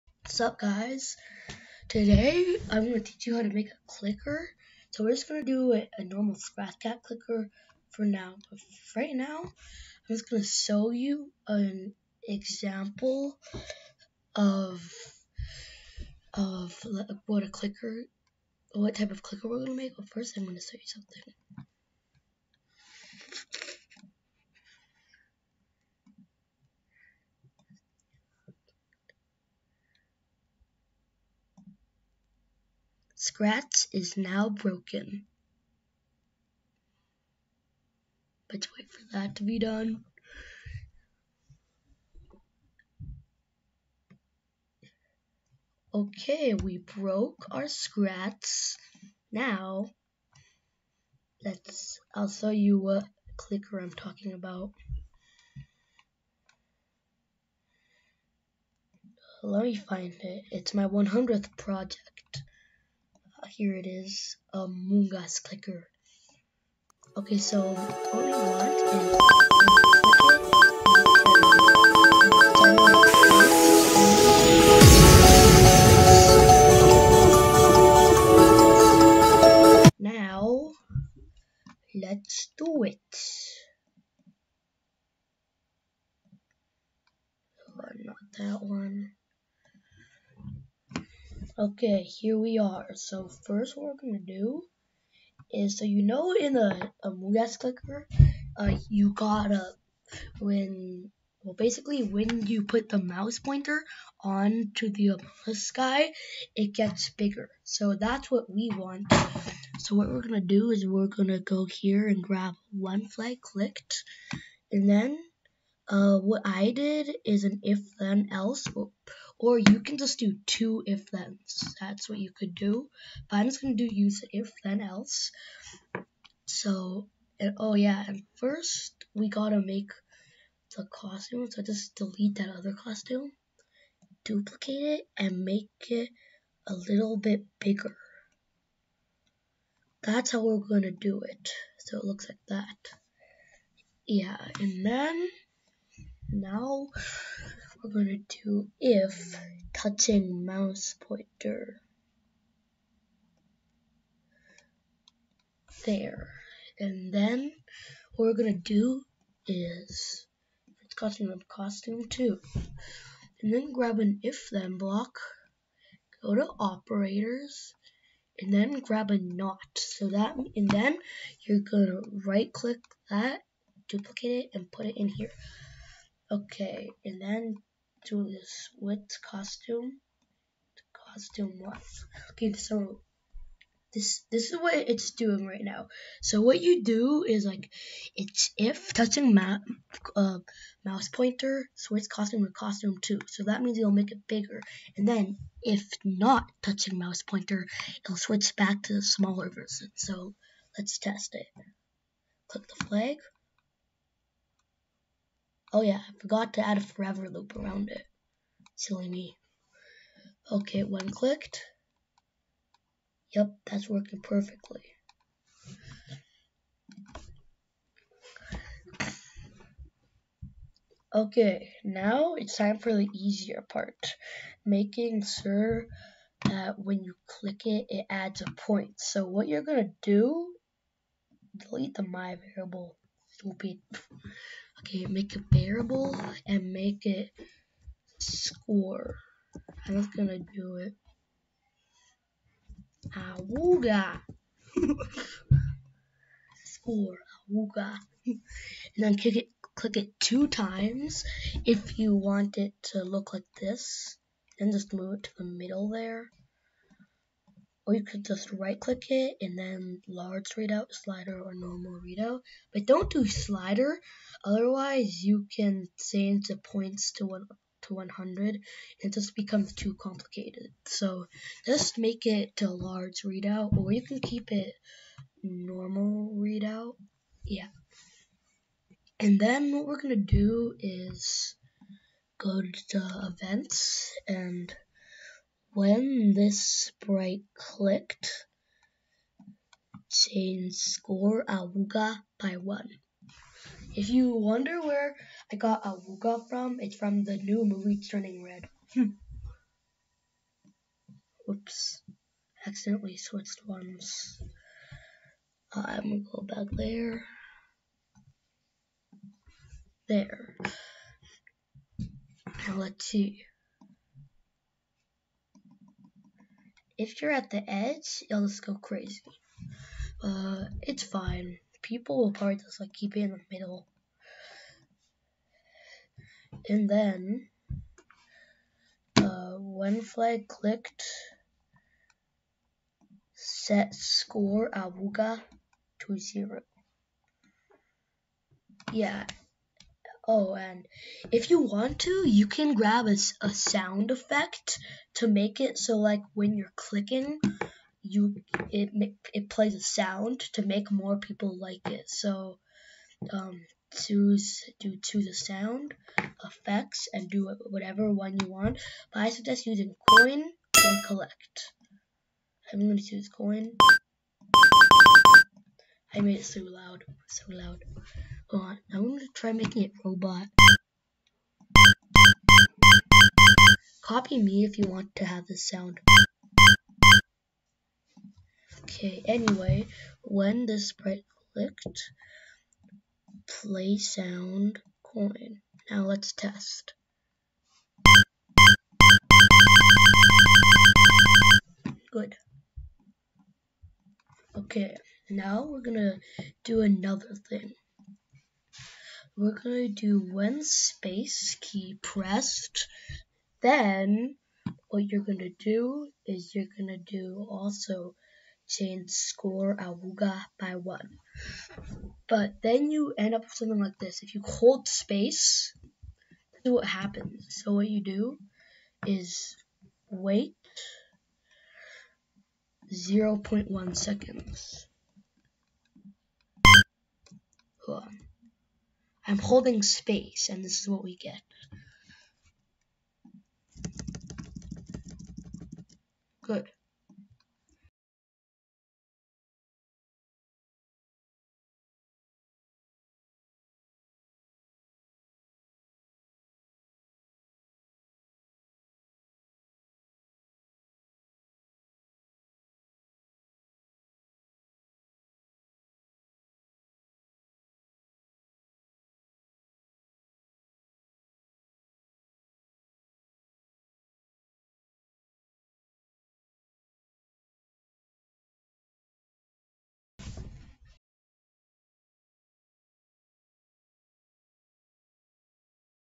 How to Make a Clicker
In this video, you will learn how to make a clicker on Scratch.